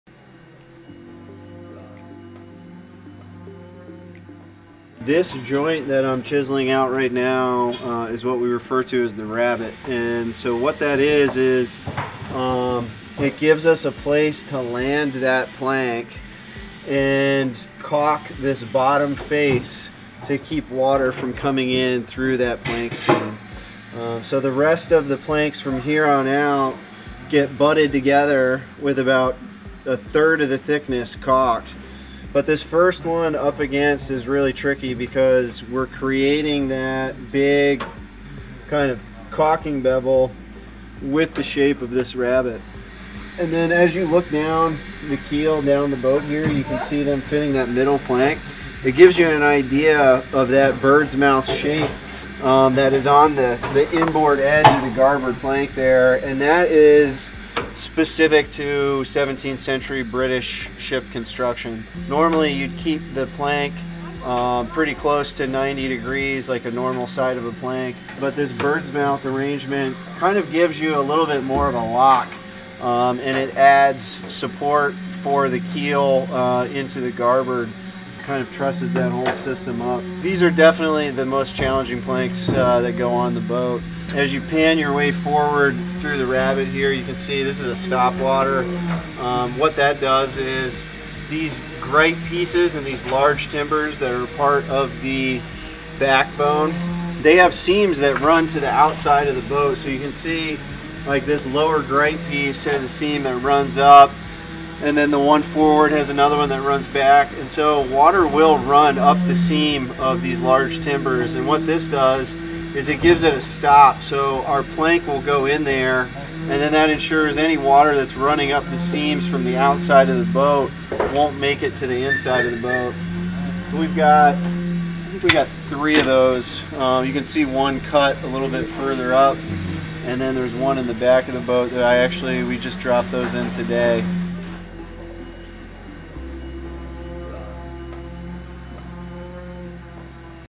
Maryland Dove: Chiseling out the Rabbet
Listen to Joe Connor, CBMM's lead shipwright, as he explains the process of chiseling out the rabbet, a technique used throughout seventeenth century British shipbuilding!

Video by Blink Horn Productions